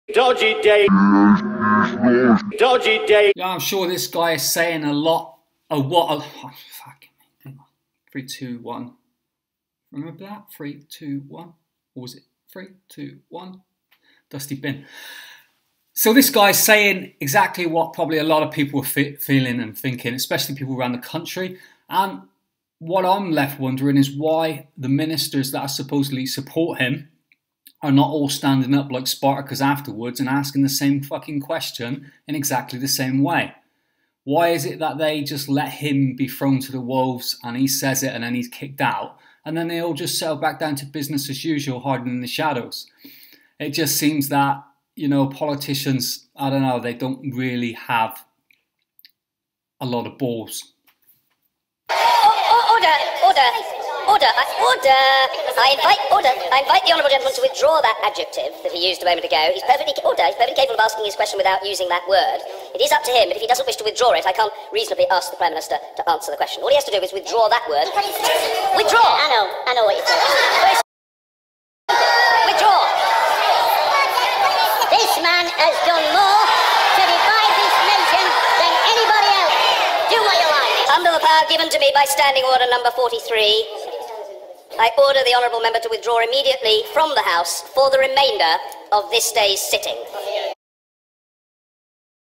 Denis Skinner And Dodgy Dave In Westminster
But wheres the support?

Denis Skinner calls David Cameron Dodgy Dave and receives no support. Why?